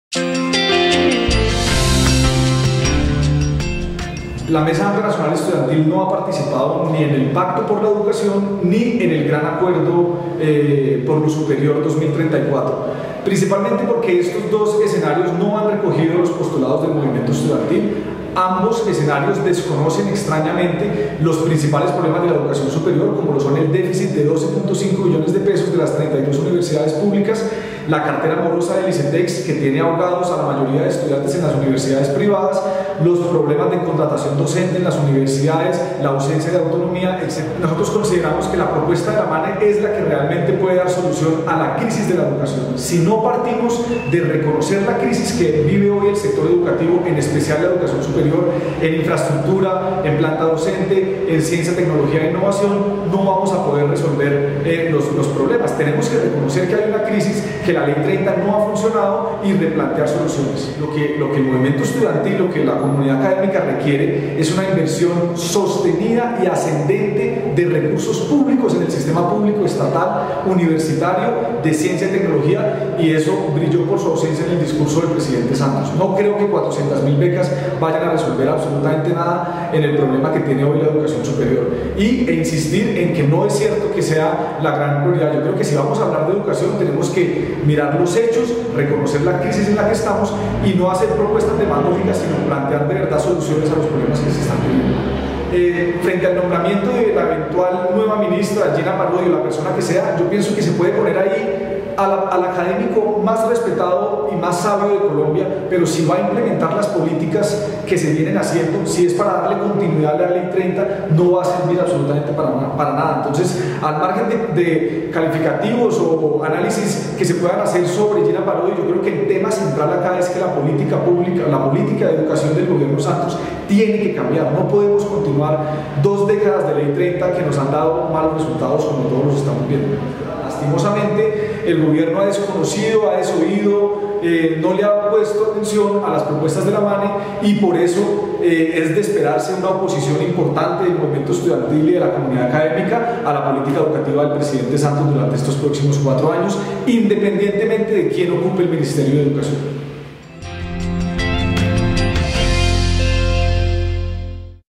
La MANE y los acuerdos por la educación
Juan Sebastian López vocero de la Mane, habla sobre la oposición por parte de este movimiento estudiantil al “Pacto por la educación” y al ‘Acuerdo por lo Superior 2034´. Para ellos, la política de educación no puede seguir dando continuidad a  dos décadas de la crisis generada por la Ley 30.